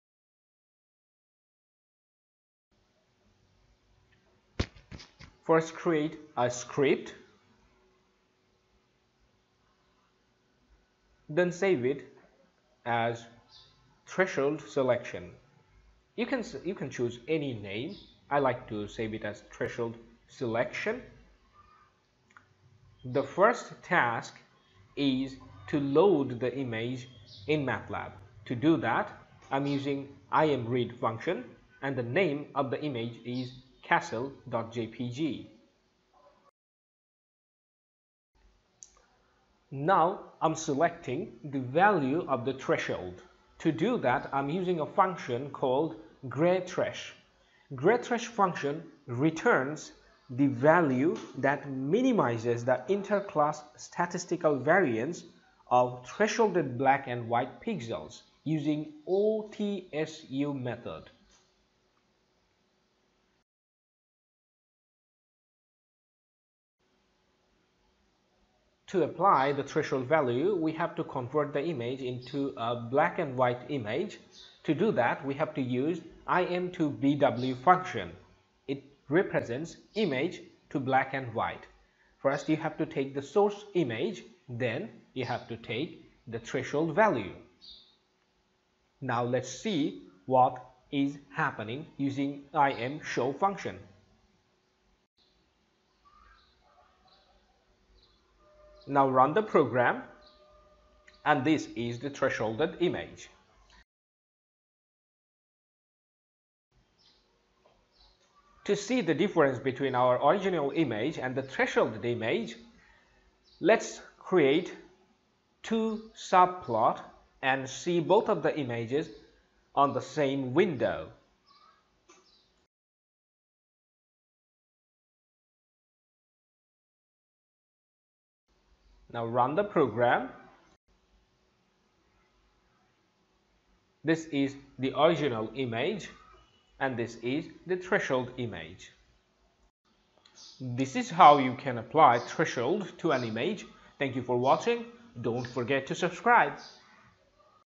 Lesson 22: Threshold Selection for Image using MATLAB
Threshold selection for image using matlab - how to apply a specific threshold value to entire image.

In this tutorial, we are going to learn how to apply global threshold value to an image. It means the same threshold value to all of the pixels. I have used OTSU method to calculate the threshold value that minimizes the inter-class statistical variance of thresholded black and white pixels.

I hope this quick tutorial will help you if you ever get stuck in applying threshold value to an image. Here is the code used in this tutorial -

image = imread('castle.jpg');
threshold_value = graythresh(image);
threshold_image = im2bw(image,threshold_value);

subplot(1,2,1),imshow(threshold_image);
subplot(1,2,2), imshow(image);

You can copy, modify, share or you can do anything else with this code snippet.